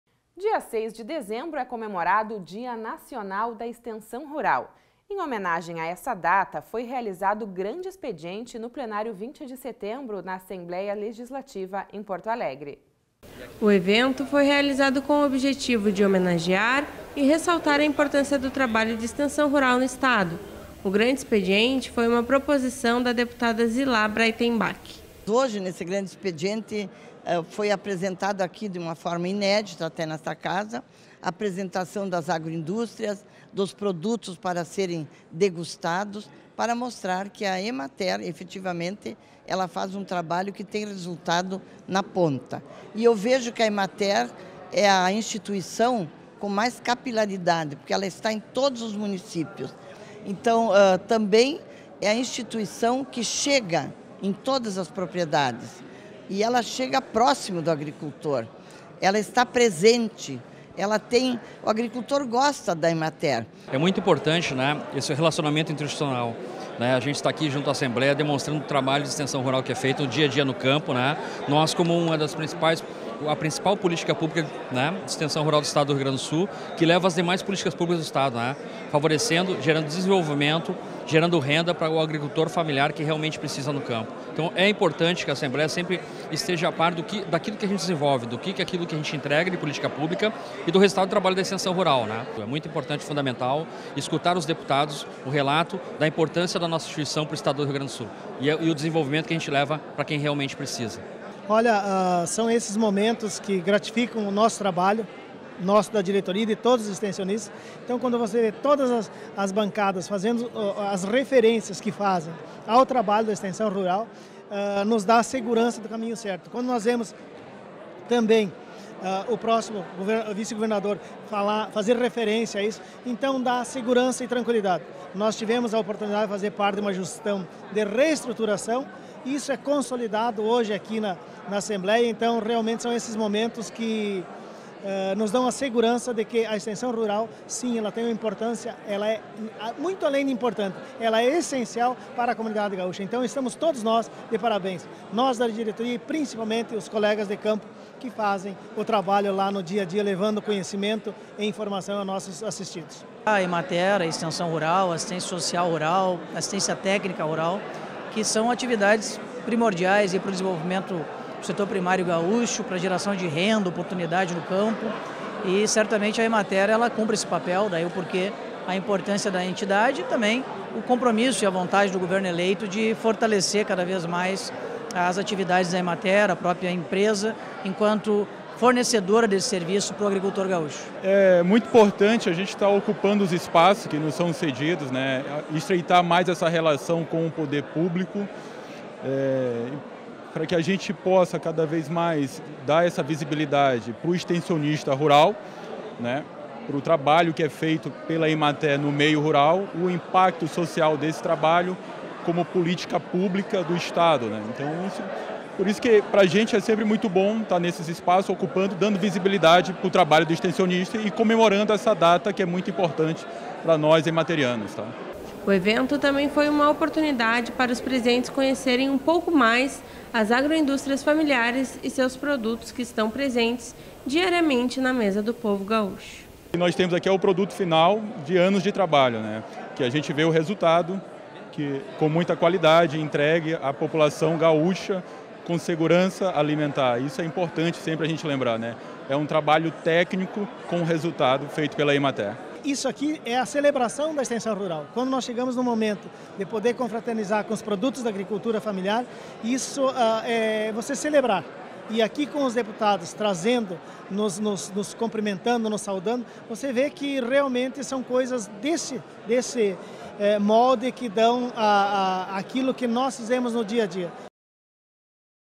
Dia Nacional da Extensão Rural - Grande Expediente na Assembleia Legislativa do RS -Rio Grande Rural
Dia 06 de dezembro é comemorado o “Dia Nacional da Extensão Rural”. Em homenagem a essa data, foi realizado Grande Expediente no Plenário 20 de Setembro da Assembleia Legislativa, em Porto Alegre/RS.